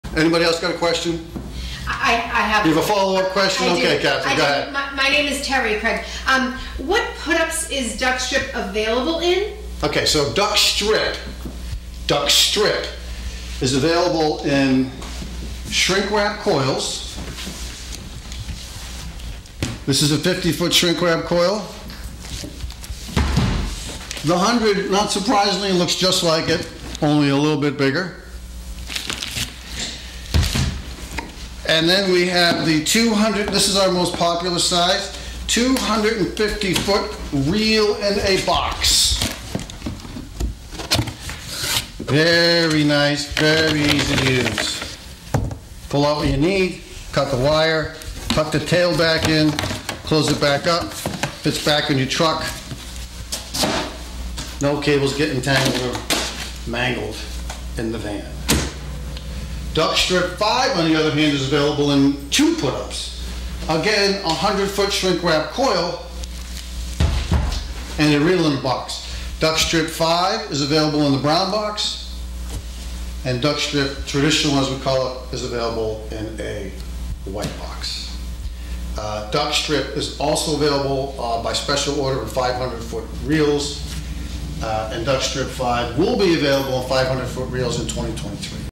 What 'put ups' is Duckt-Strip and Duckt-Strip 5 available in?
Duckt-Strip Press Conference Q&A. for more videos and information.